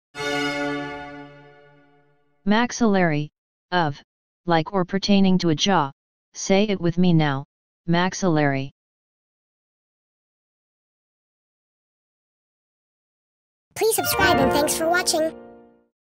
maxillary - Smart & Obscure English Words Defined 👁️🔊🗣🧠✅
This video is about "maxillary". Here's a video of some of the most obscure English words with definitions that will definitely make you sound smart (more smarter) if used properly.